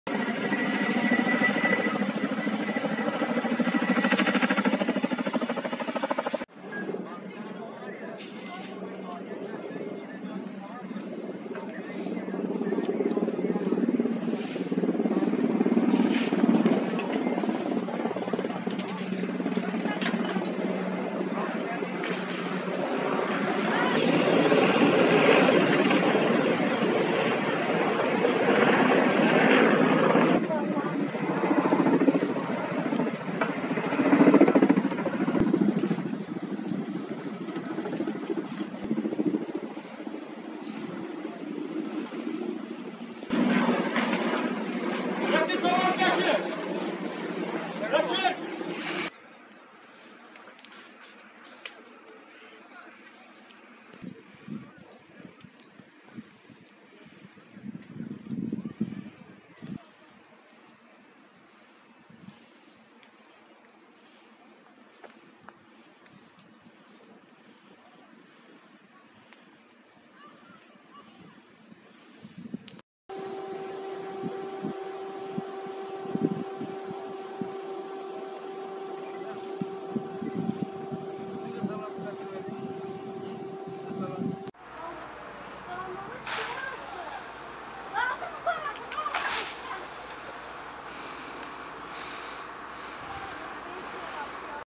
SEYDİKEMER DE YANGIN
TEMİZLİK ATEŞİ 30 HEKTAR ZEYTİNLİK ALANI KÜL ETTİ
Seydikemer İzzettin mahallesinde, tarım arazisinde tamamen söndürülmeden bırakılan temizlik ateşinden kaynaklı çıkan yangında, 4 hektar orman ile 30 hektar tarım arazisi yandı. Yerleşim yerlerine de zarar veren yangın havadan ve karadan müdahele ile 3 saatte güçlükle söndürüldü.